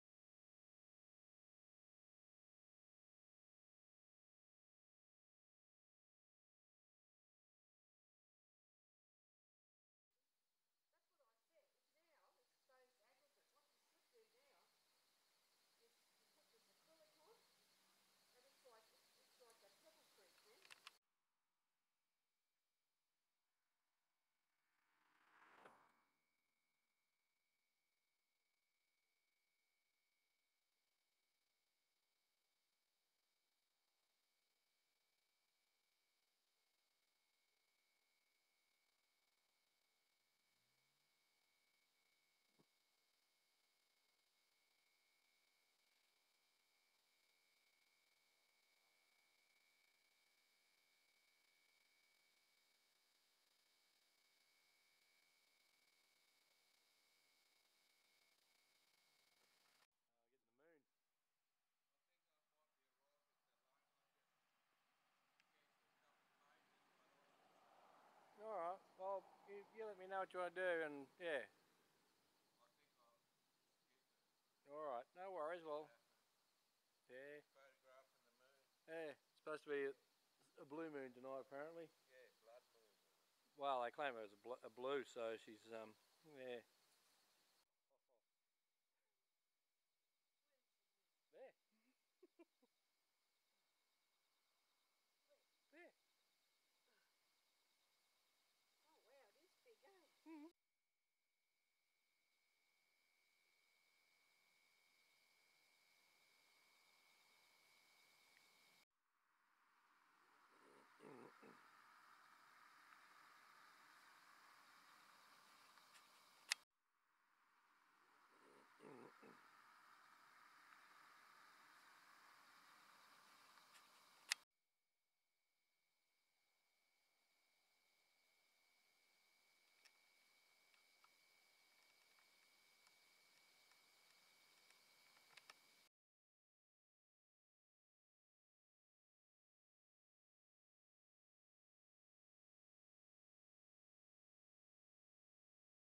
Blue Moon 31st August 2023
Well!! It's here!! The long expected Blue Moon of August 2023. Using my now outdated as it seems, Sony 4K capable HDR Camcorder, I bring to you my capturing video of tonight's spectacular event?? The footage is quite clear, and the characteristics of the moon can be clearly seen. We even see it change to a yellowy - pinky color at times, before going white. Be some time before we see this again?? Enjoy and goodnight!!